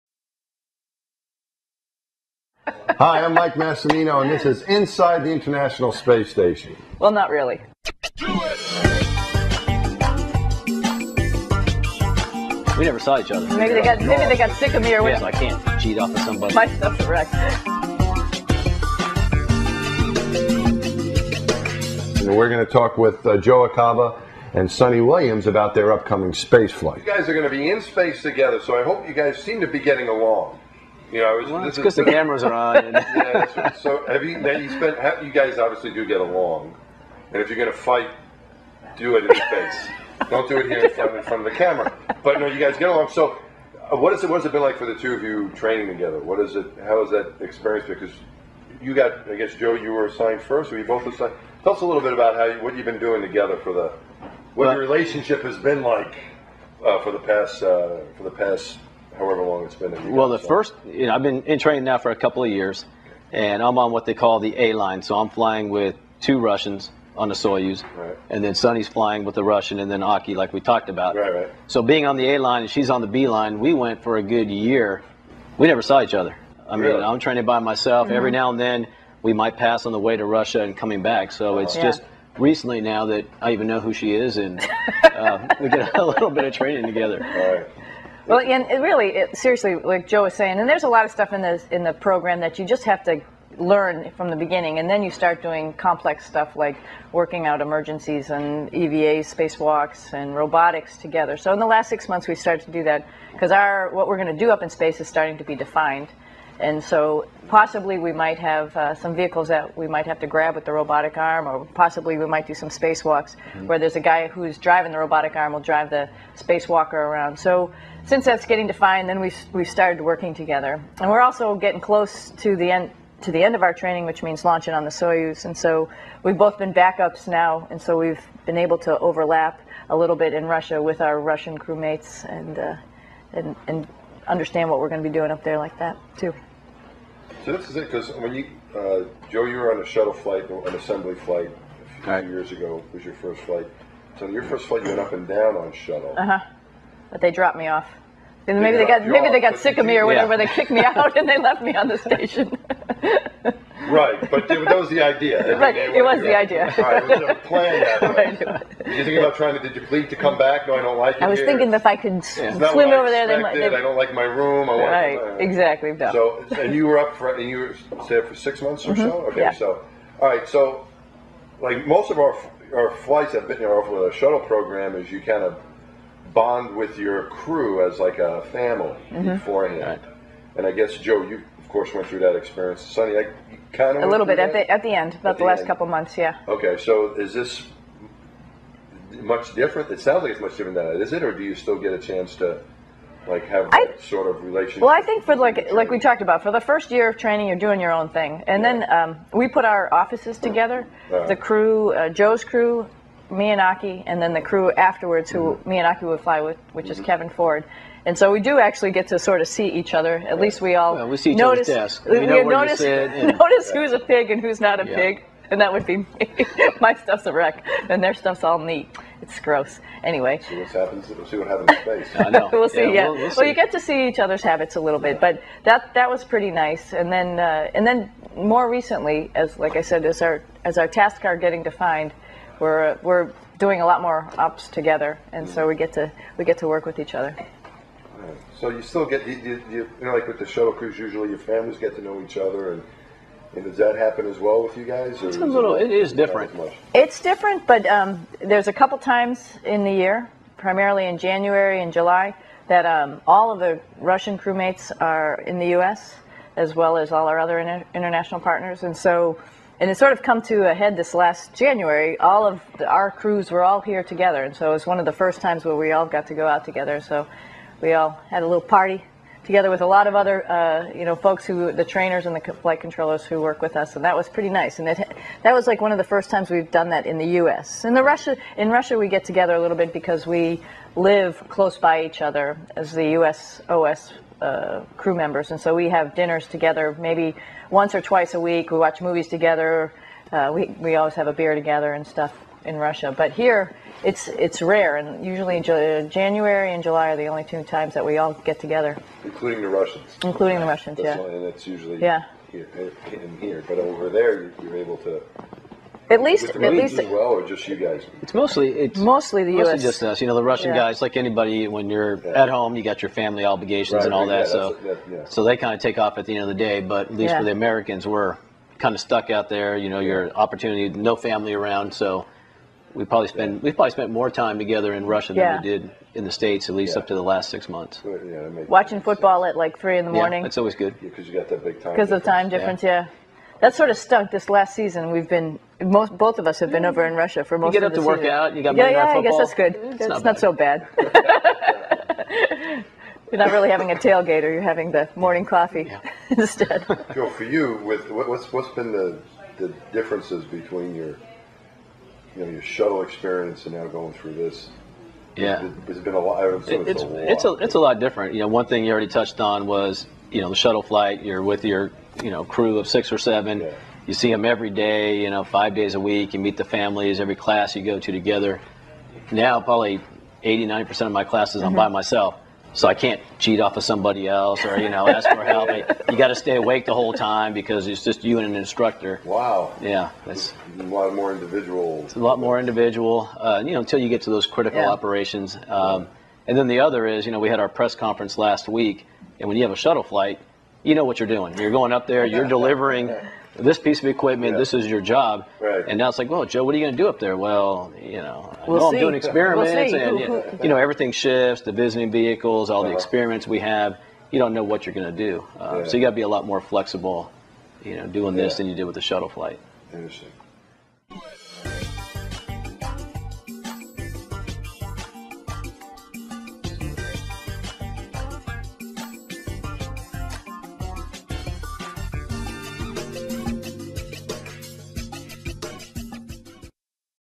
Inside the ISS: Training Together
NASA astronauts Suni Williams and Joe Acaba discuss the training flow necessary to prepare for a mission aboard the International Space Station.